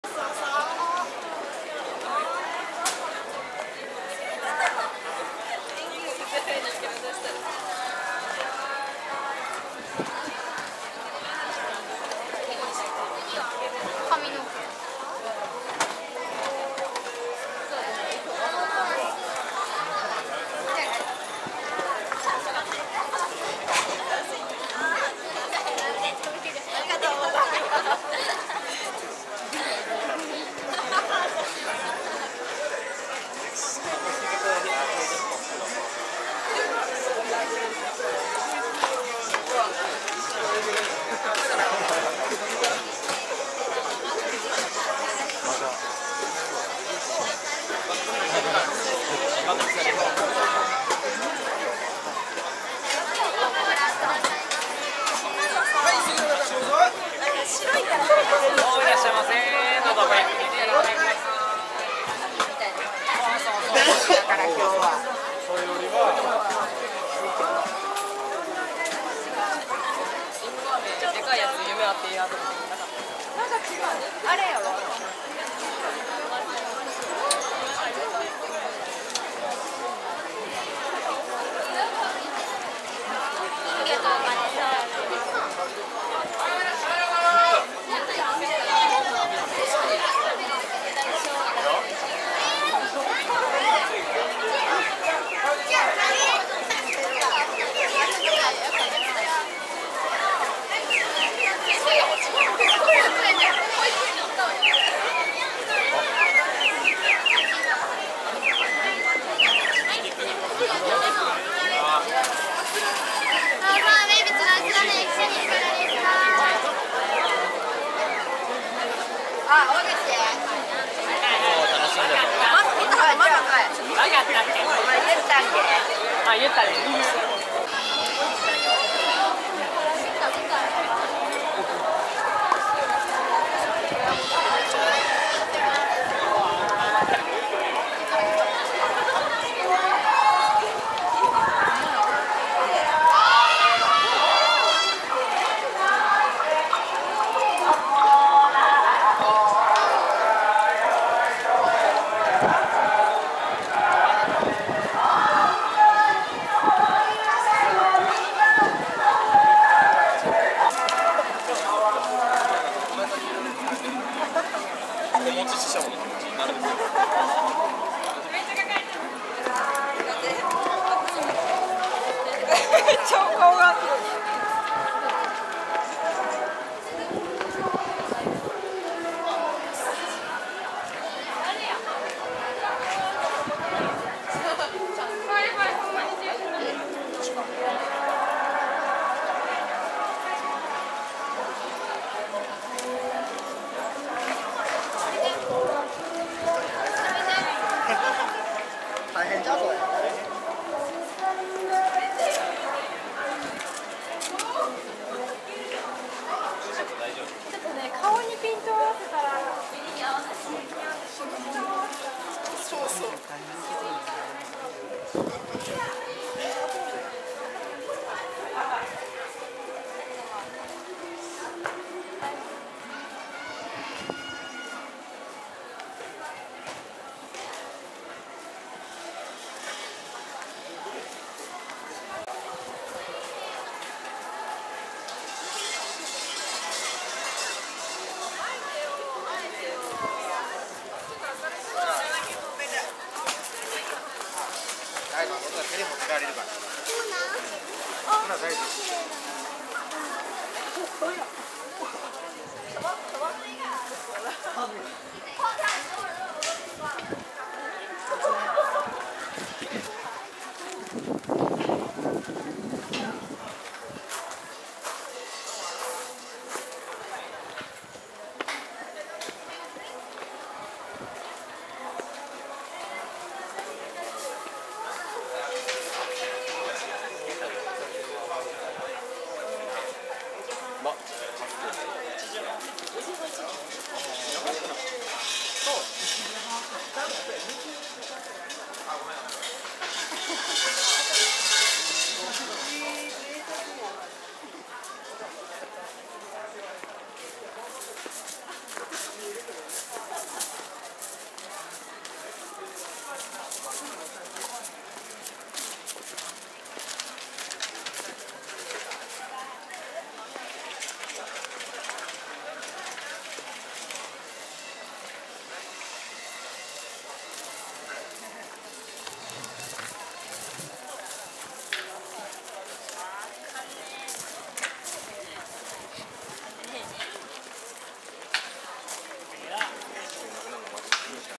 伏見稲荷大社 本宮祭2016 - その1
2016年7月25日 京都 伏見稲荷大社で行われた、本営祭
境内にたくさんの提灯が並びライトアップされ、幻想的な世界が広がります。
撮影機材
 ソニーアクションカム：FDR-X3000(4Kモード)